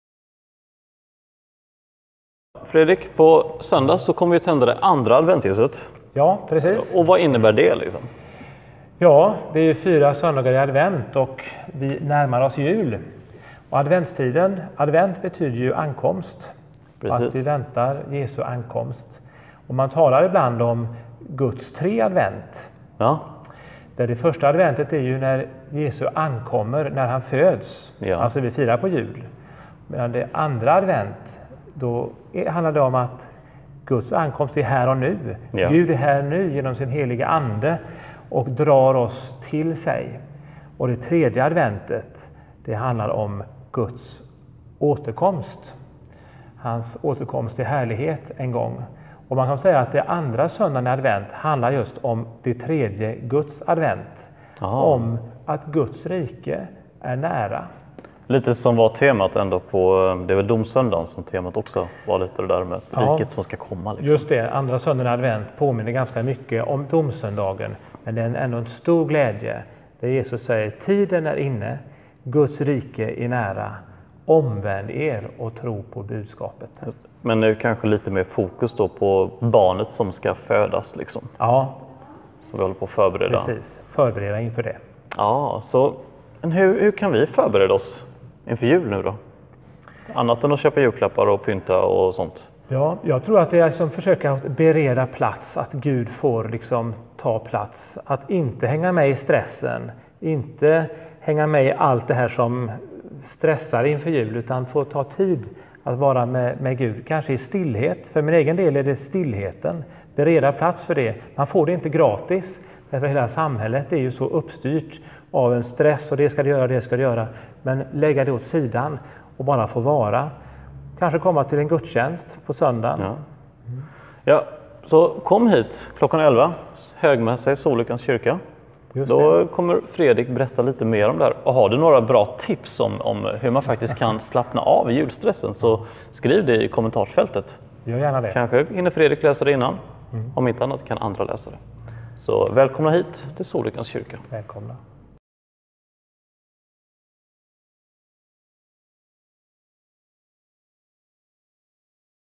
Inför söndag 4 december 2016
Fredrik Gyllensvärd funderar lite kring Jesus som ankommer på olika sätt. Han ger även några tips i julatressen.
Söndag 4 december 11:00 är det Stilla mässa i Sollyckans kyrka.